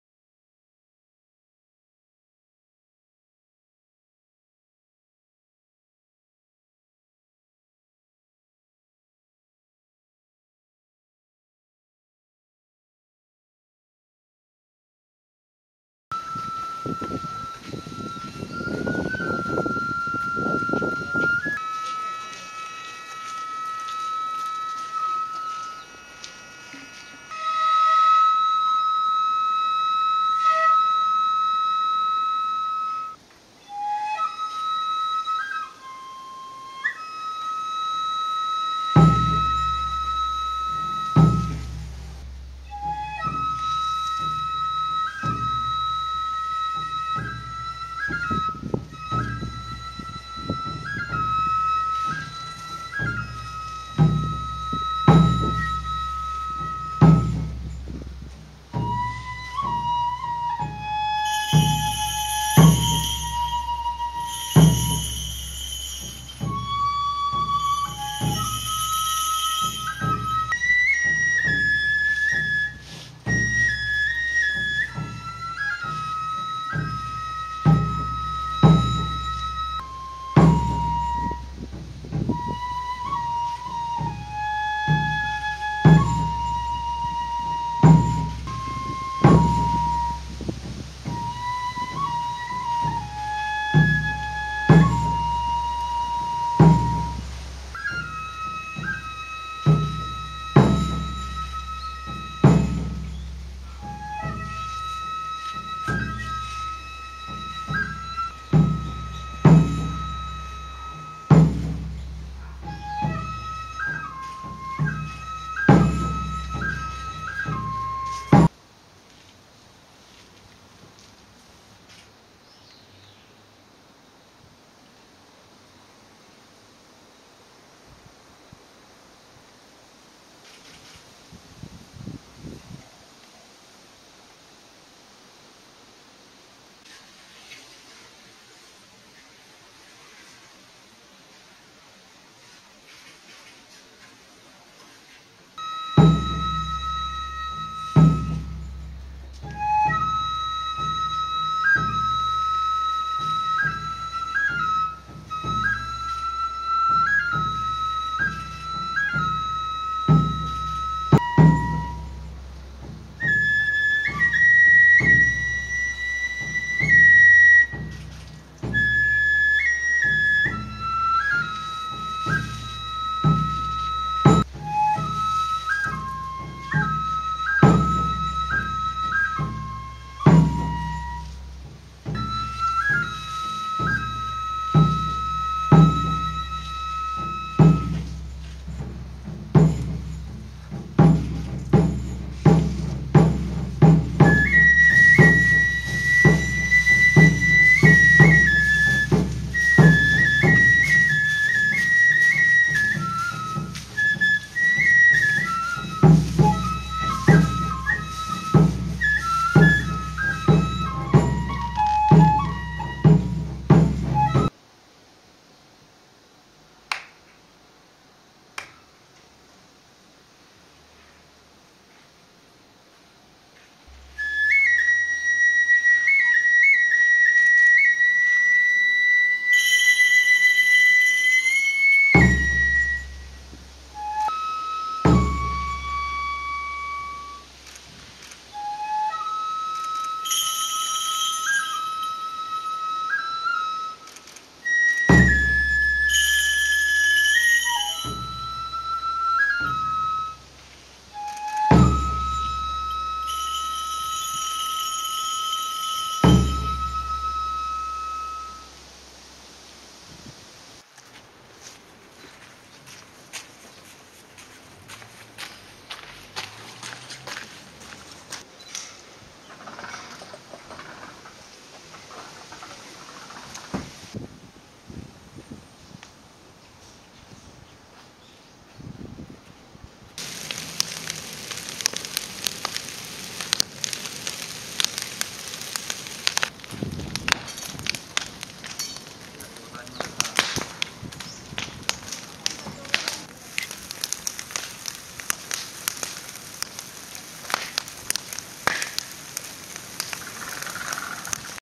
長野神社 大松明神事
奥河内・河内長野市長野神社の大松明神事です。この大松明神事は市指定の無形民俗文化財の一つであり、毎年10月11日に催されているものですが、今年の松明は規模を小さくして井桁を組んで行われていました。雅楽が奏でられる中、本殿の前で神事がとり行われ、続いて湯立神事と鈴の舞が奉納されました。続いて松明に火が入れられ、300年以上の歴史ある松明神事が無事に今年もとり行われました。長野神社の大松明神事っていいな！奥河内っていいな！